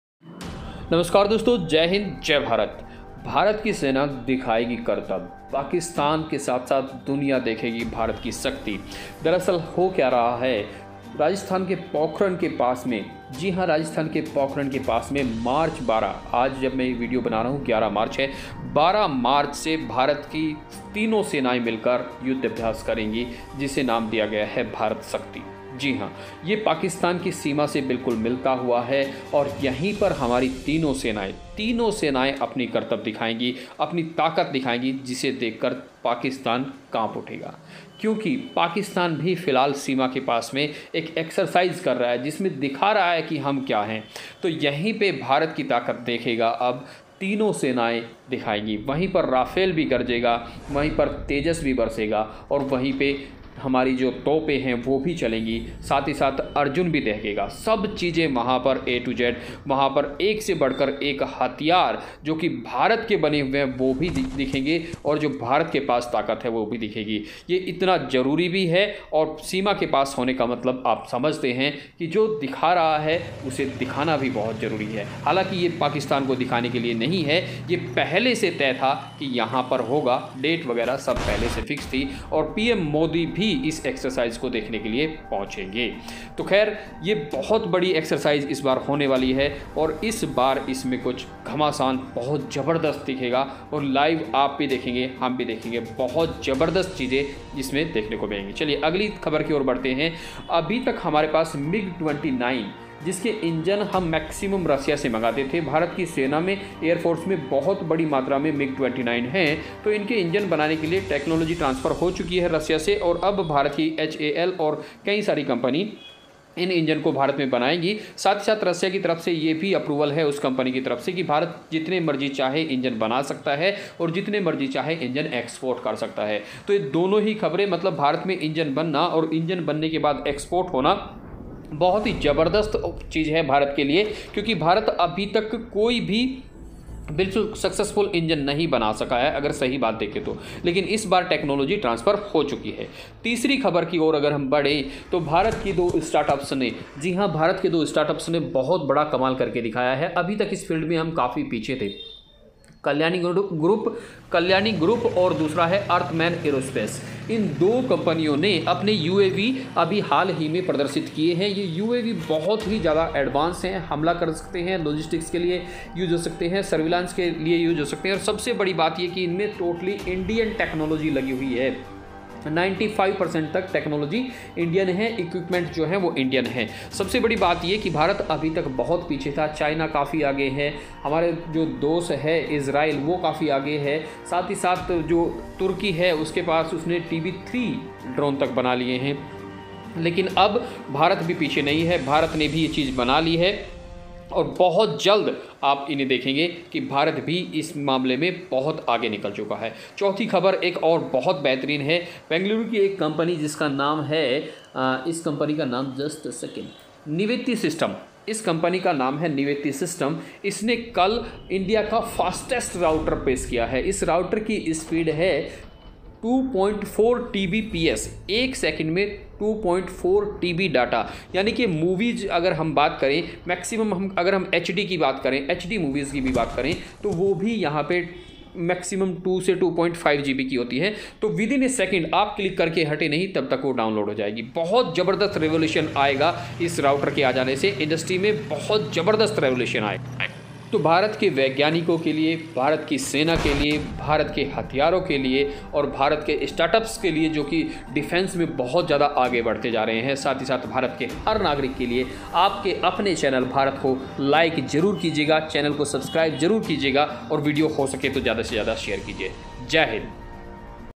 गरजेगा राफेल, आग बरसाएगा अर्जुन , भारत शक्ति देखेगा पाकिस्तान
In this video you will hear about our upcoming military exercise Bharat Shakti. and our two Indian startups show advanced UAV . Nivetti System launches India,s fastest router .

Rafale
military exercise
tank
Pakistan
china
drone
uav
router
India